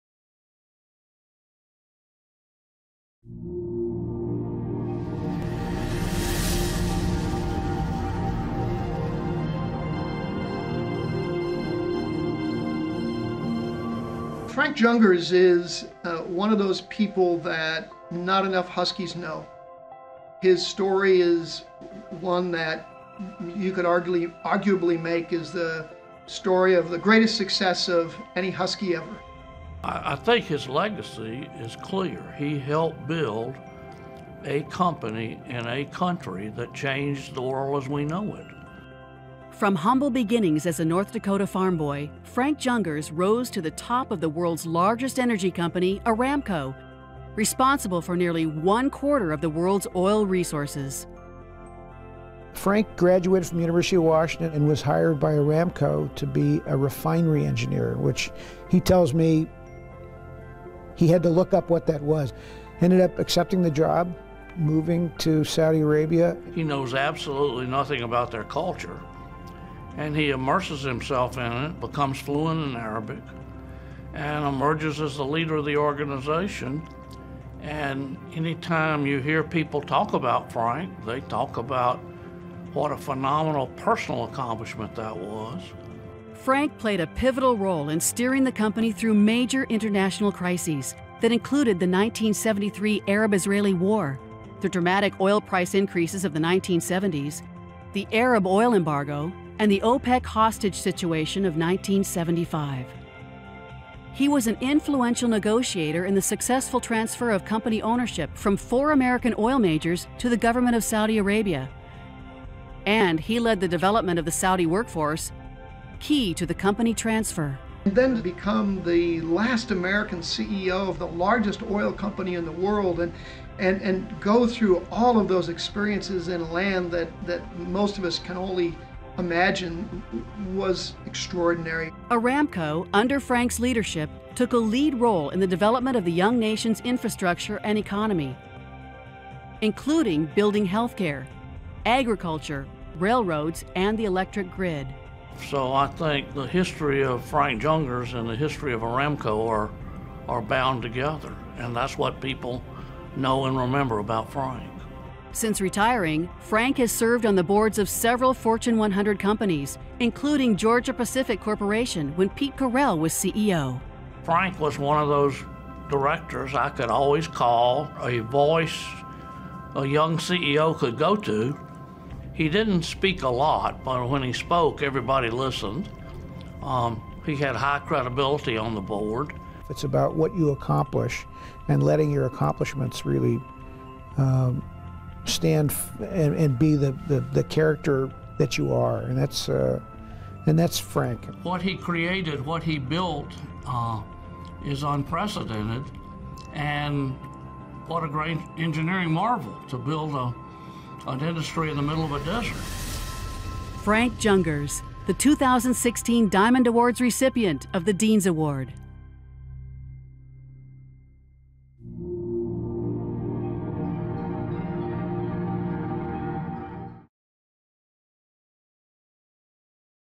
Diamond Awards 2016: Frank Jungers
Frank Jungers, '47 BS Mechanical Engineering
Chairman and CEO (ret.), Aramco
Dean's Award

During a turbulent time in the Middle East, Frank Jungers was an influential figure in board rooms, in halls of government and, occasionally, on the media circuit. Having ascended the ranks at the petroleum giant Arabian-American Oil Company (now Saudi Aramco), Frank served as a delegate, spokesperson and decision-maker during negotiations for the transfer of Aramco ownership from the U.S. to Saudi Arabian government.